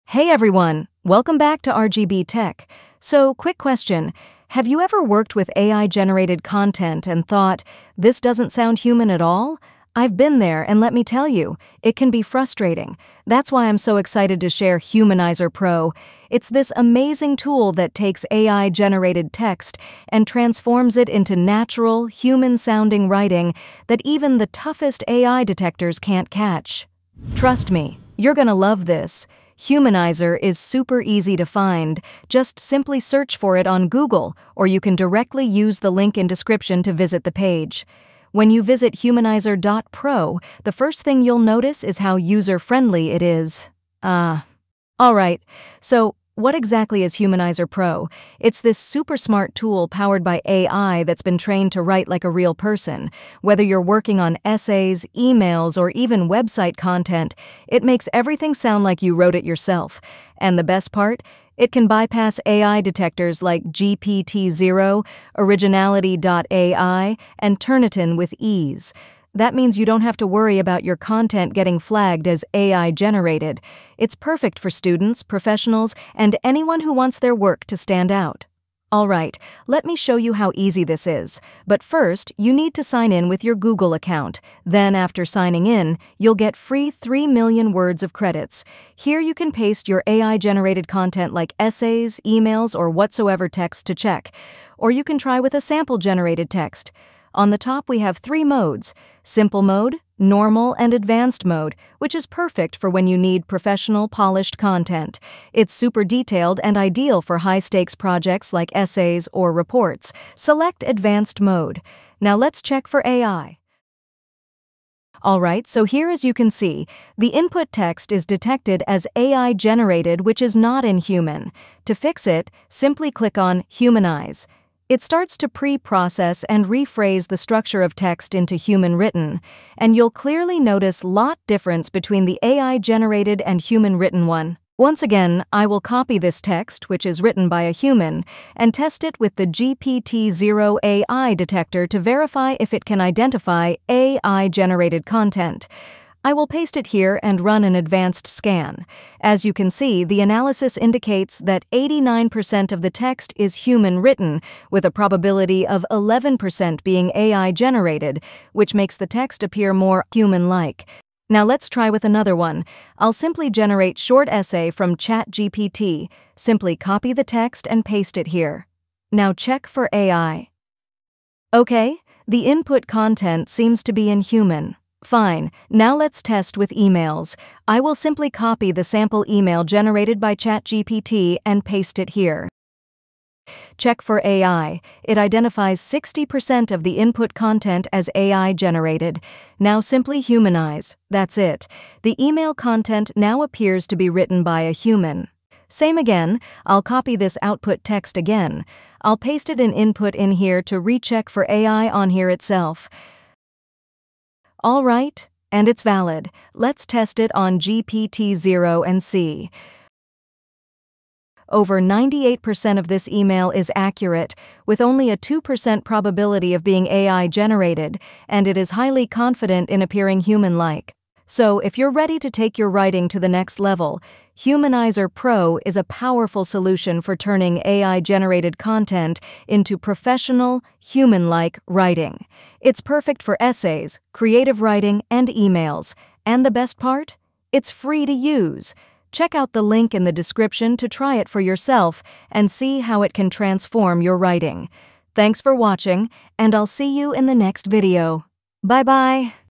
The Best Free AI Text Humanizer You Need to Try | Bypass AI Checkers - Humanizer.Pro
Want to make your AI-generated content sound truly human? In this video, I’ll show you how to use Humanizer.Pro to bypass AI detection and create professional, human-like writing effortlessly. Whether you’re working on essays, emails, or creative writing, Humanizer.Pro offers three powerful modes—Simple, Normal, and Advanced—to fit your needs. And yes, it’s completely free to start!

📌 100% Free! Try humanizer now